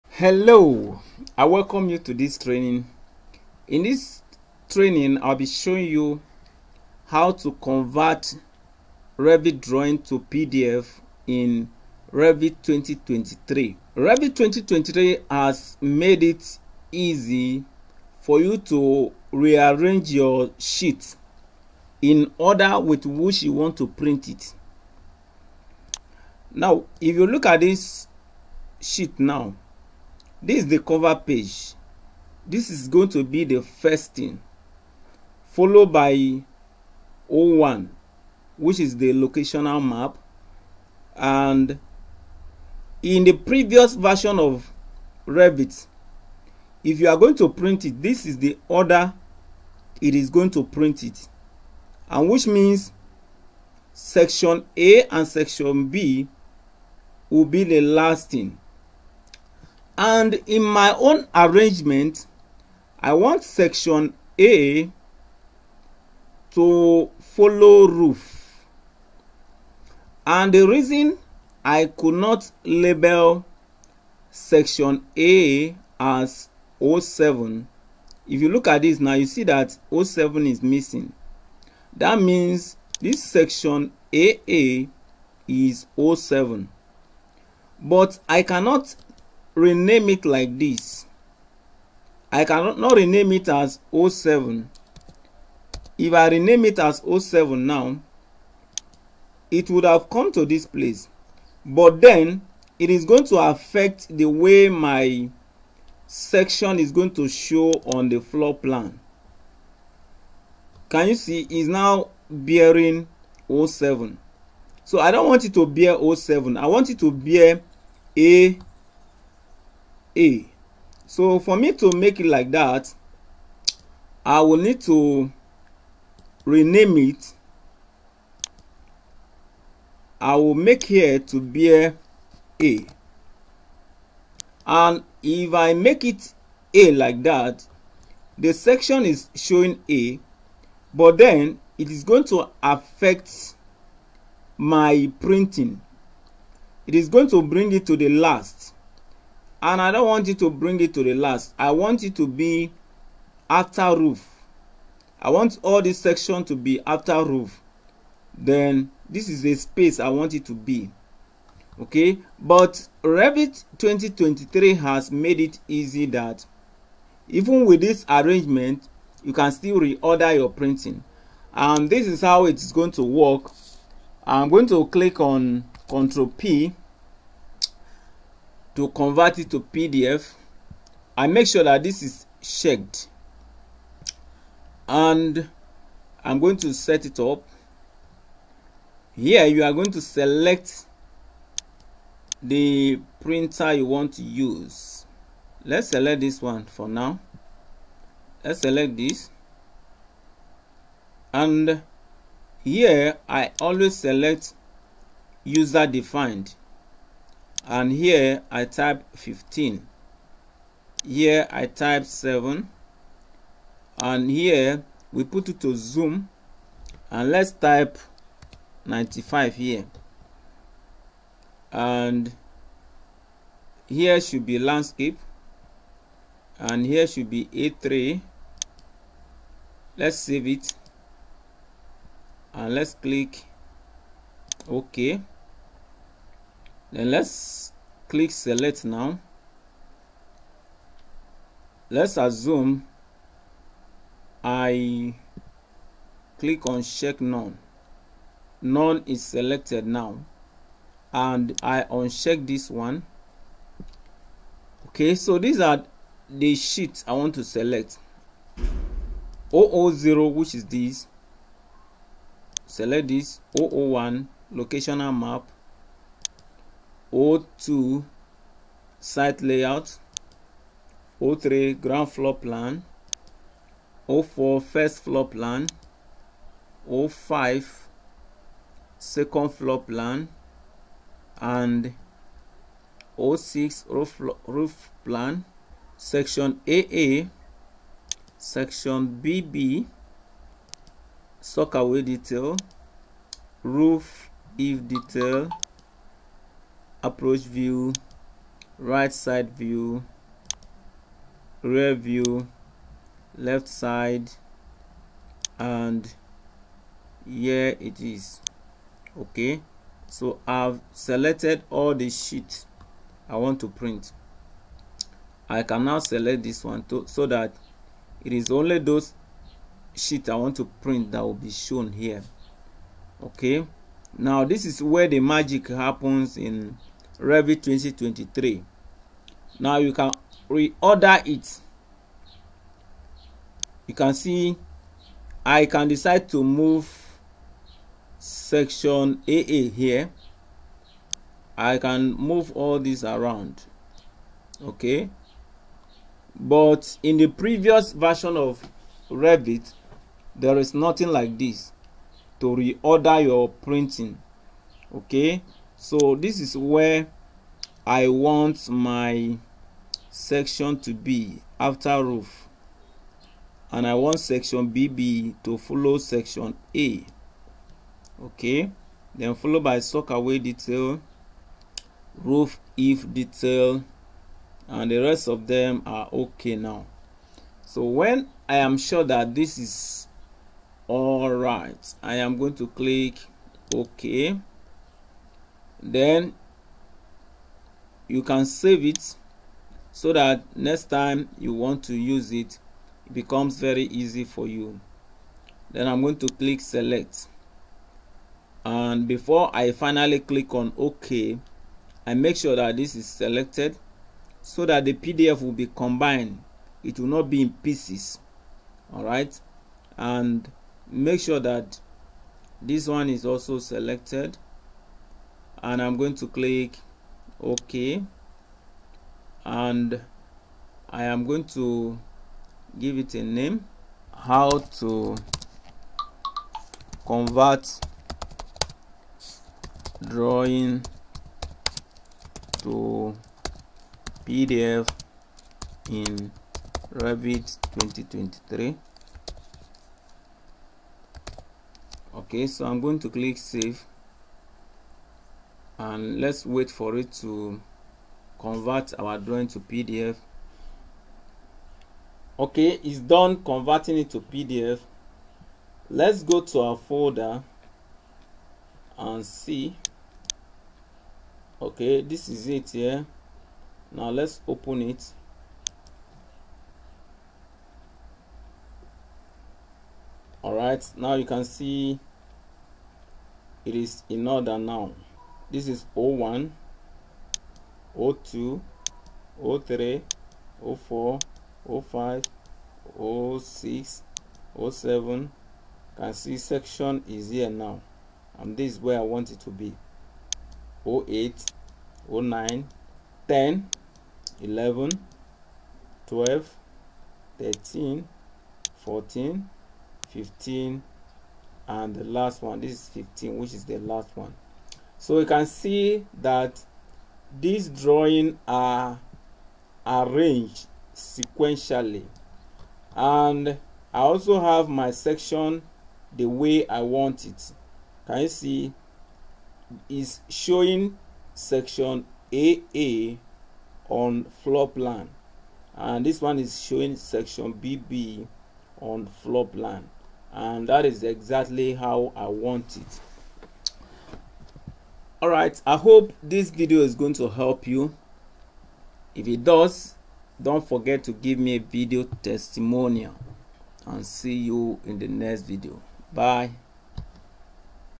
How to Convert Drawing to PDF in Revit 2023
This video shows how to Convert Drawings to PDF in Revit 2023.